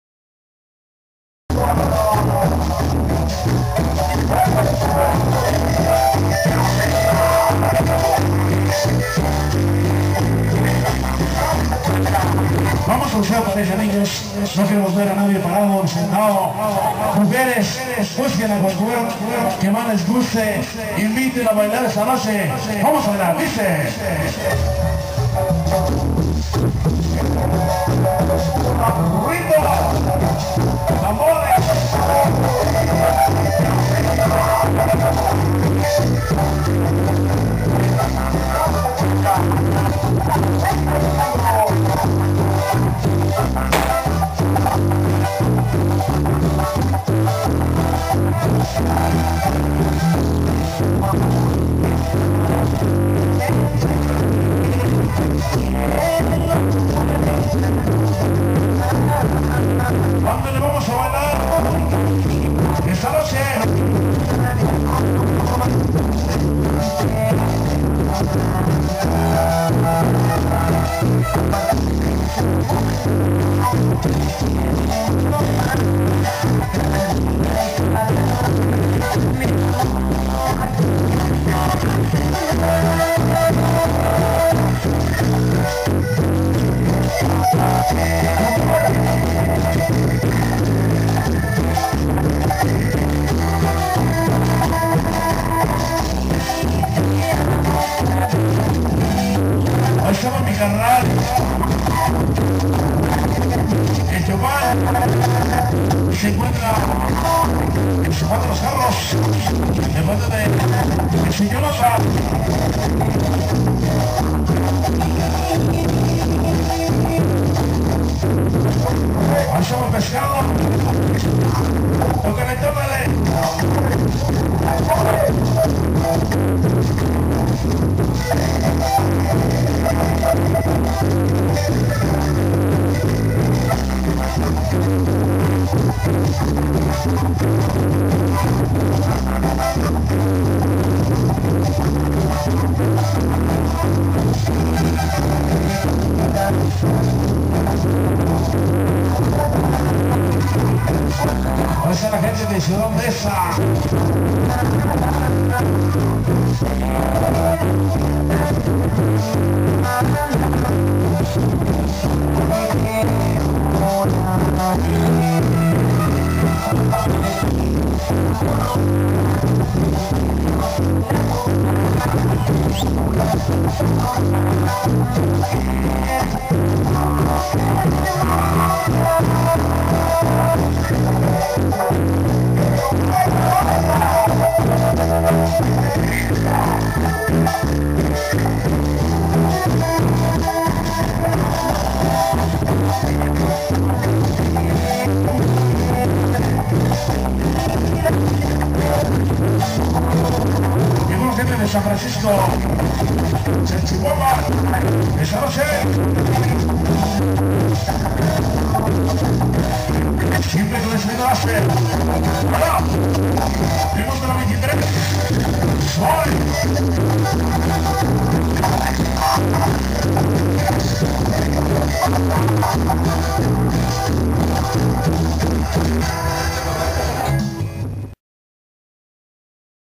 Sonido LASSER-San Pedro del Rosal
Set de sonido Lasser, desde Santa Maria Nativitas Tecoac hasta san pedro del rosal, saludas a la familia Miranda,familia Becerril y familia Claudio,a los novios, me trae varios recuerdos este lugar muy buenos recuerdos, saludos a la prepa 134, secundaria no. 160, a los chuntaros de la L29,los chicos solitarios y a la bandera de san francisco chalchihuapan...Para ti Elia